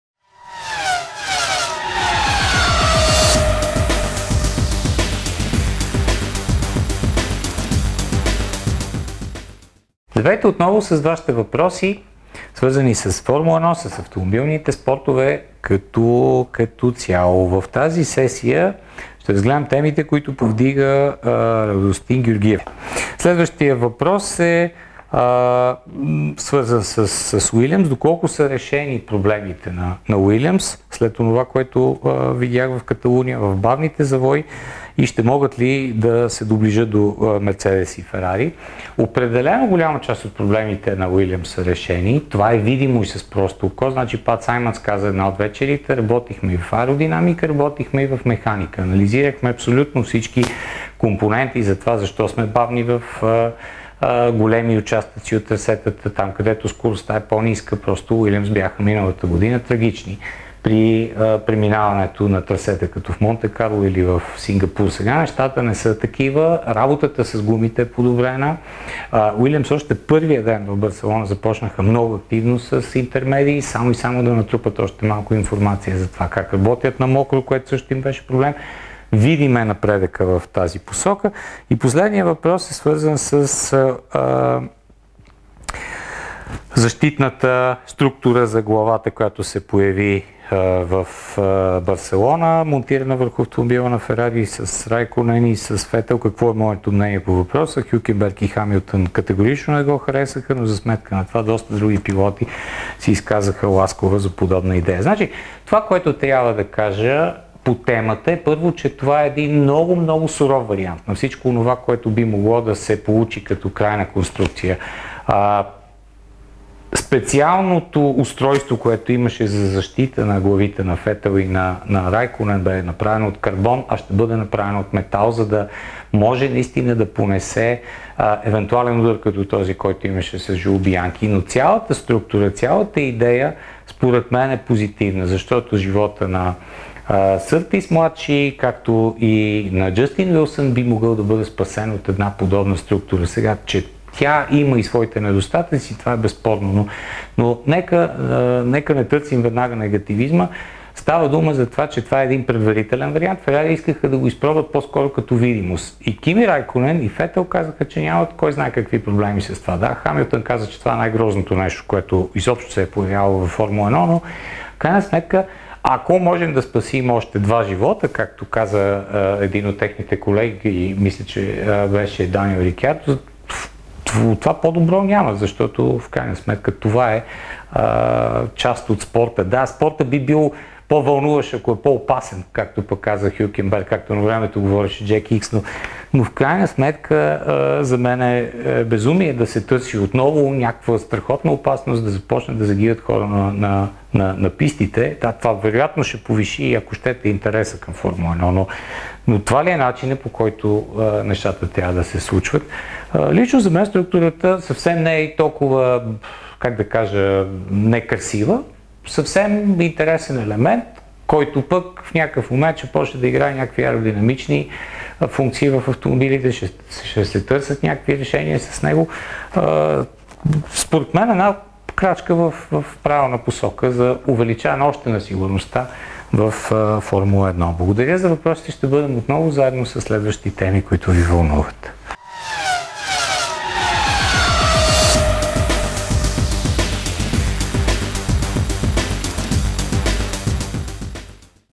Решиха ли си проблемите в Уилямс?
Въпрос на Радостин Георгиев:
Решиха ли Уилямс, според вашите наблюдения от пистата "Каталуния" проблемите си в бавните завои от предишните 2 години и смятате ли, че FW38 най-сетне е готов да съперничи, макар и в отделни надпревари, на големите акули
"Mercedes" и "Ferrari"?
Bих се радвал да коментирате i новите кокпити със защитите за главата, която бе тествана от Ферари.
Четох доста отзиви на решението, за Райконен и Фетел то е добро, за Хюлкенберг и Хамилтън то е неприемливо.
Вашият коментар и мнението Ви по тази интересна тема и няма ли според вас болидите от Гран при поне частично да заприличат на тези от LMP 1  в Le Mans?